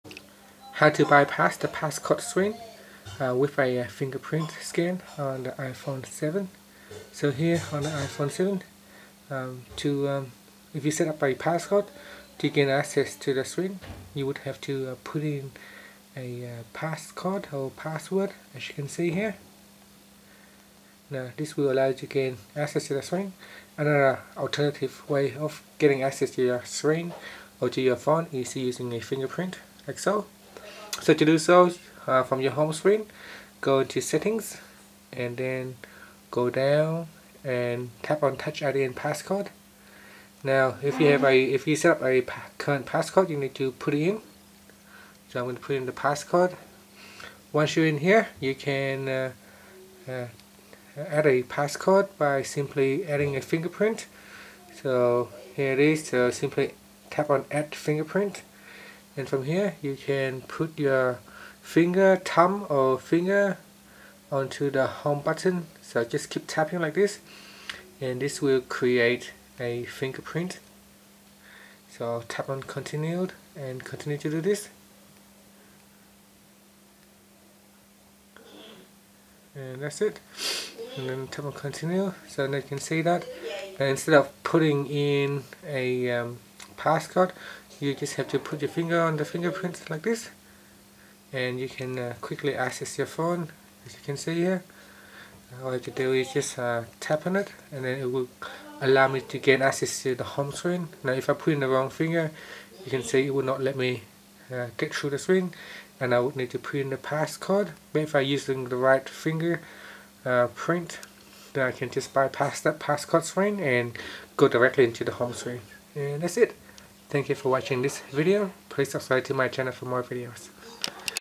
iPhone 7: How to Bypass Passcode Screen With Fingerprint Scanning
Learn how you can bypass the passcode screen with fingerprint scanning on the iPhone 7. Do you want to get access to the phone without having to put in a passcode everytime, but at the same time you want the phone to be secured? See this video for good work around.